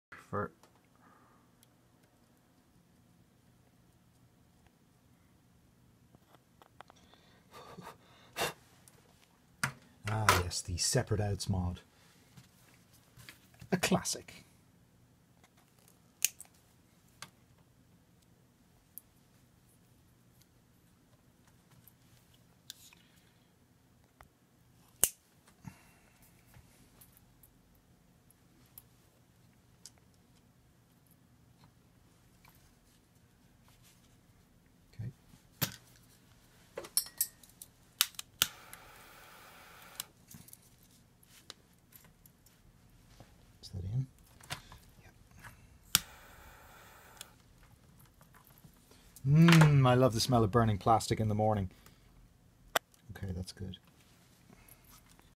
Roland TR606 Modification
Hybrasil's Roland TR606 Drum machine undergoing a modification at Mr Spring's studio.